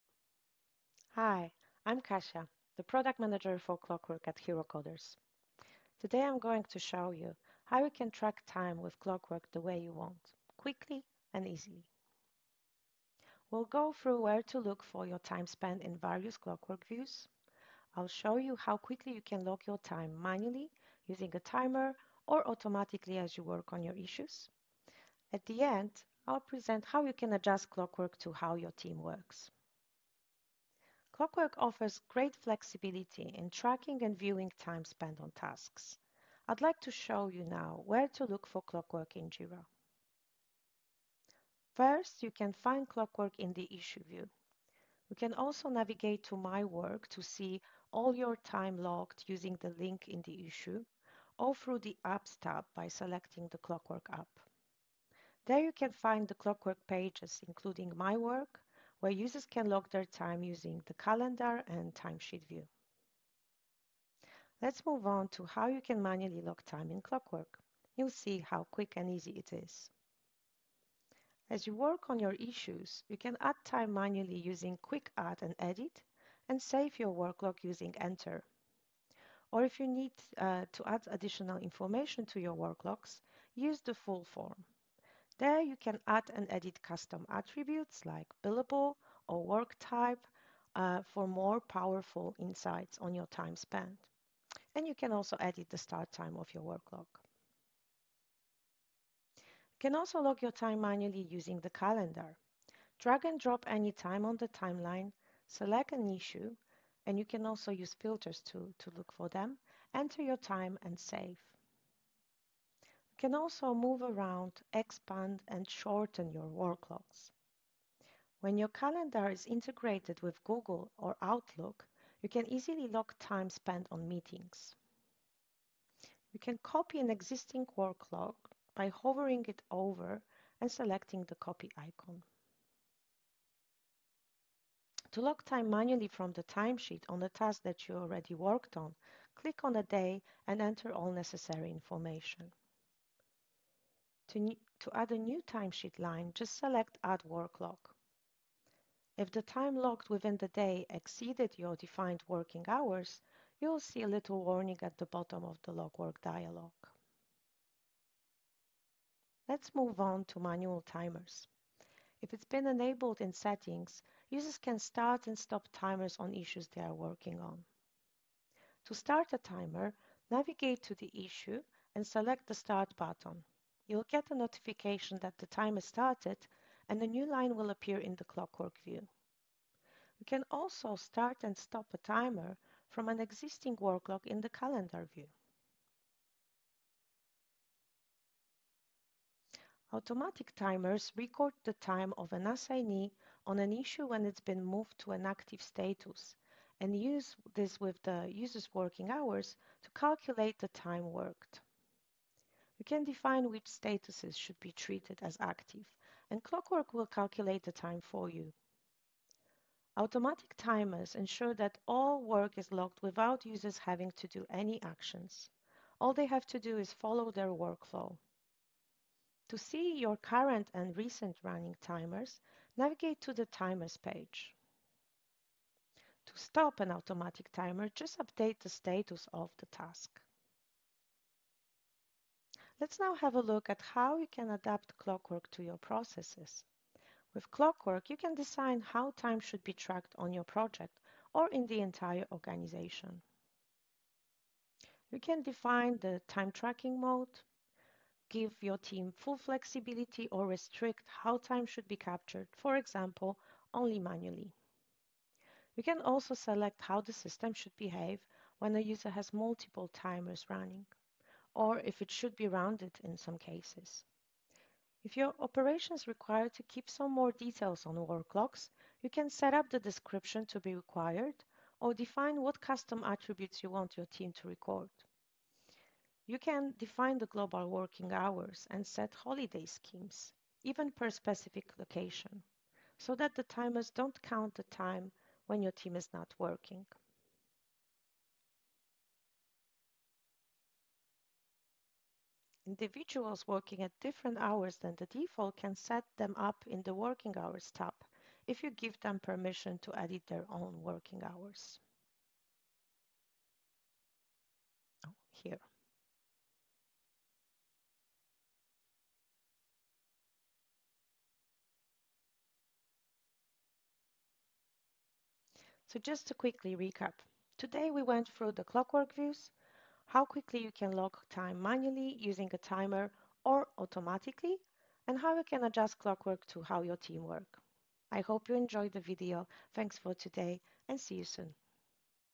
Track Time the Way You Want with Clockwork for Jira
Discover multiple ways to track time with Clockwork for Jira time tracking and timesheets solution.

Clockwork Pro is the ultimate Jira time tracking app that helps you save time, improve accuracy and increase productivity. This timesheet solution lets you track time your way with maximum flexibility.

Developed by HeroCoders, an Atlassian Platinum Marketplace Partner.